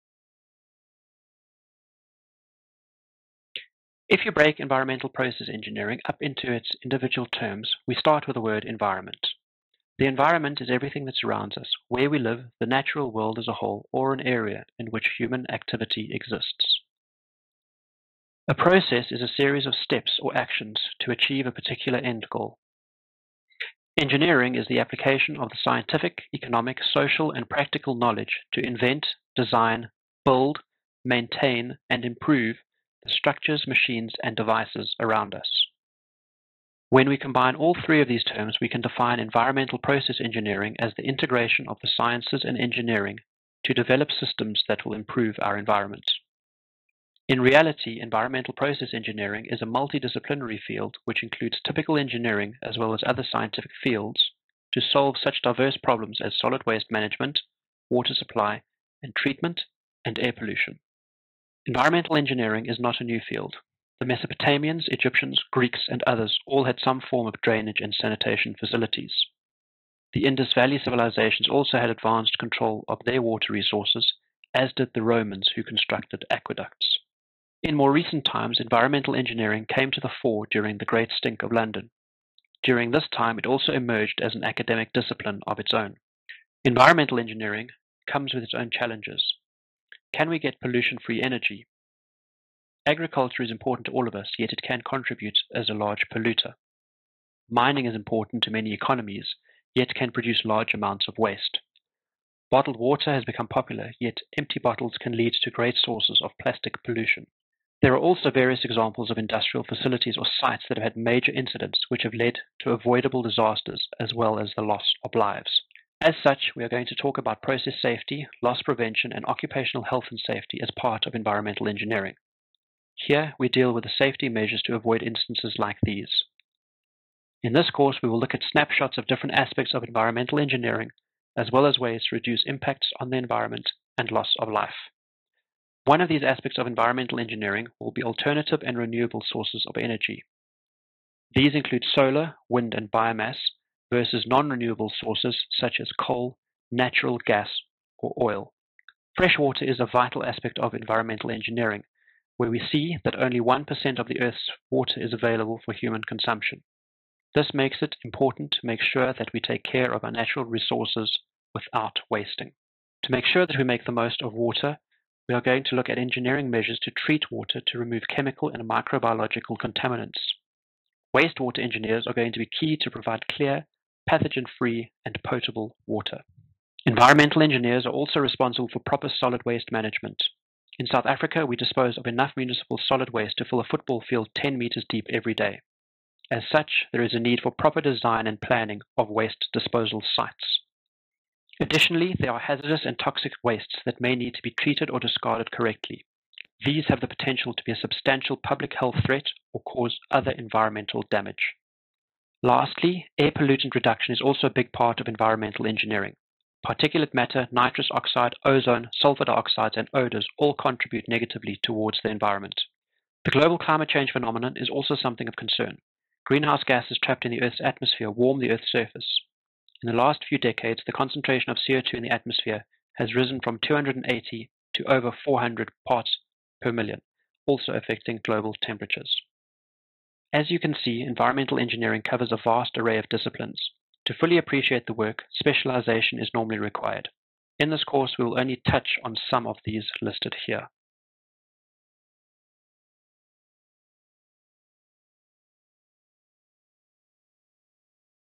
Introduction to Environmental Engineering [Lecture]
The start of a series of very (very!) shortened lecture notes for an introductory environmental process engineering undergraduate course.

An introduction to environmental process engineering.

---CONTENTS---
0:00 - Introduction
1:01 - Evolution of environmental engineering
1:26 - Environmental engineering challenges
1:47 - Environmental disasters
2:15 - Alternative and renewable energy
2:28 - Water
2:56 - Waste disposal
3:21 - Air pollution & global warming
3:49 - Fields of specialisation

As taught at the University of the Witwatersrand, Johannesburg, School of Chemical and Metallurgical Engineering.